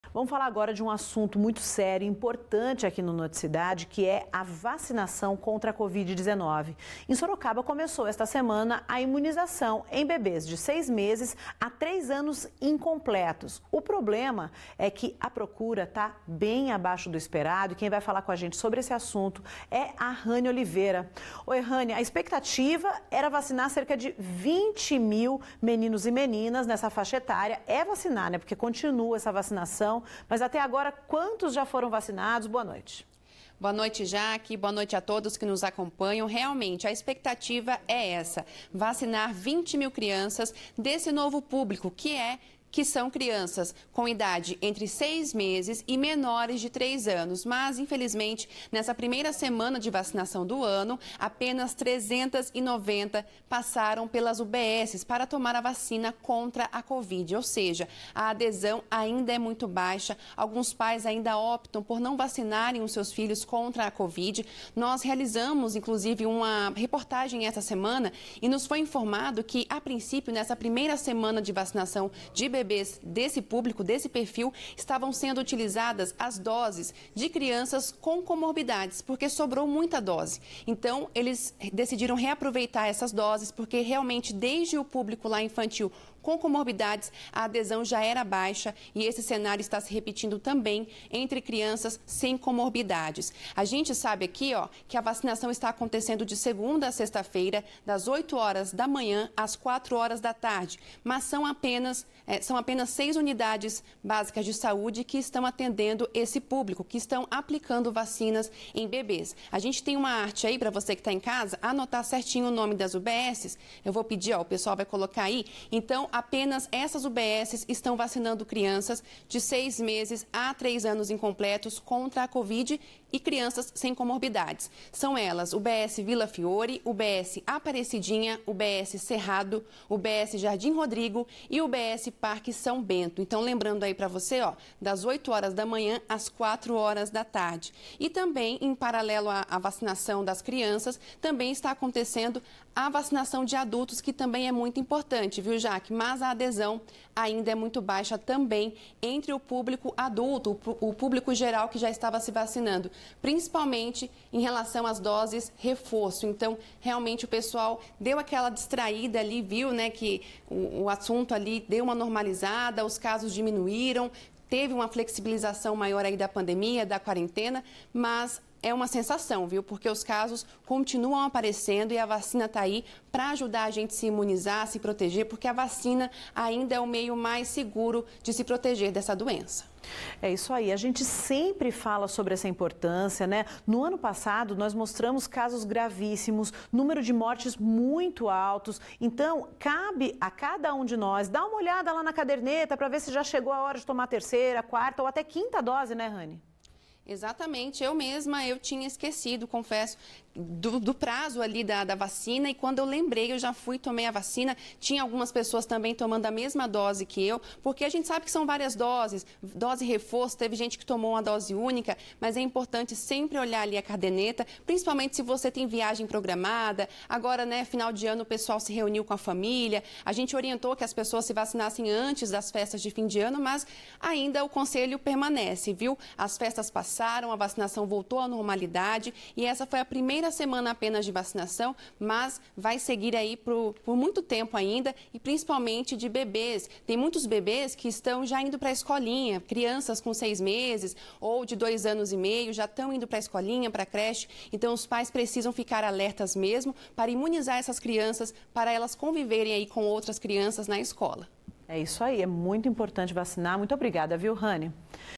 Sorocaba tem baixa adesão na vacinação de bebês | TV Sorocaba SBT
Sorocaba começou a vacinar bebês de seis meses a dois anos e 11 meses sem comorbidades nesta semana, mas a adesão desse público está abaixo do esperado